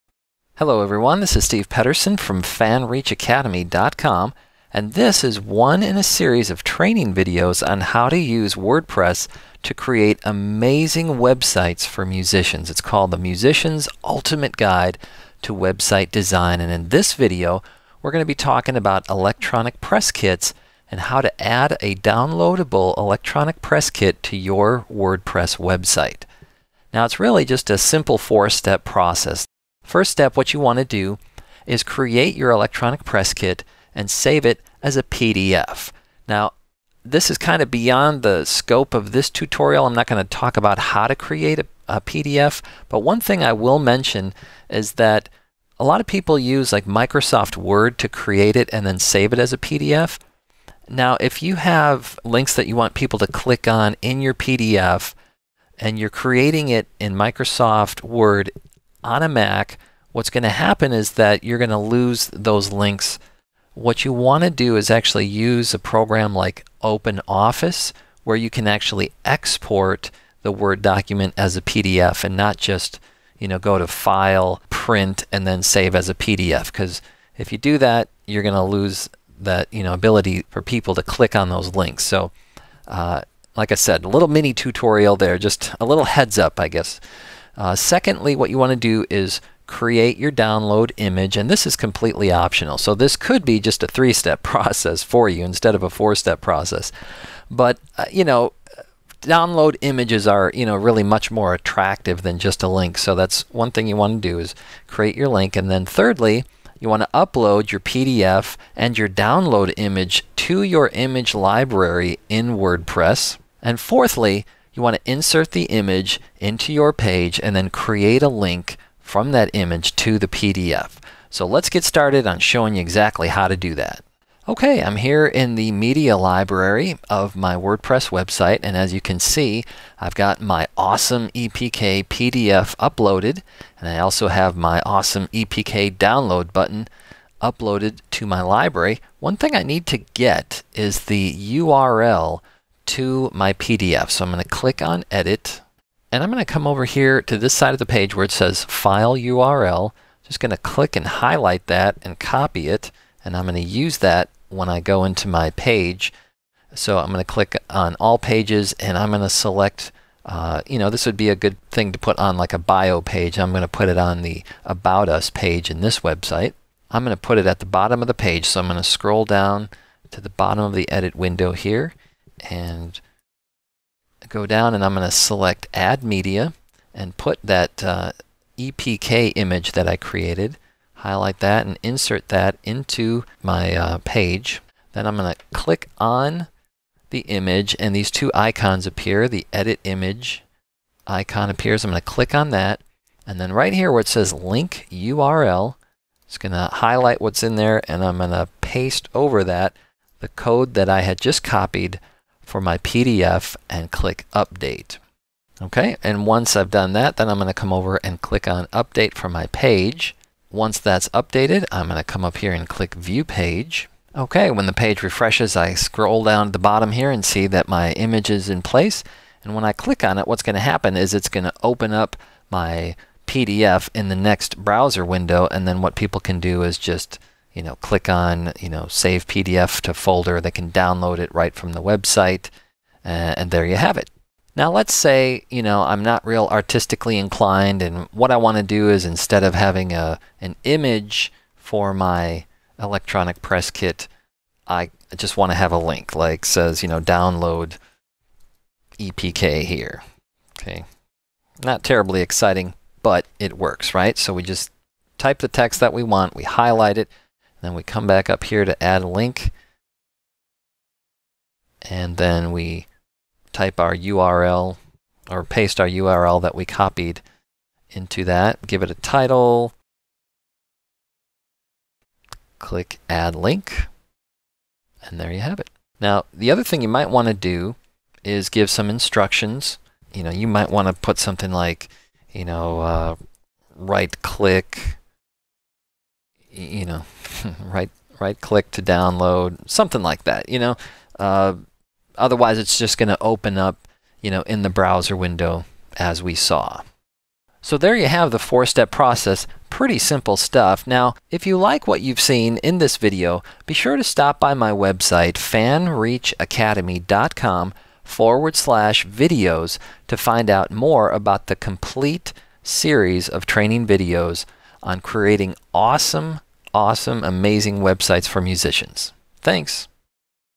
How to add an Electronic Press Kit to Your Wordpress Website
to learn more about this complete series of tutorial videos.

In this video Steve Pederson of Fan Reach Academy demonstrates how to add a downloadable PDF version of an Electronic Press Kit (EPK) to your Wordpress Website.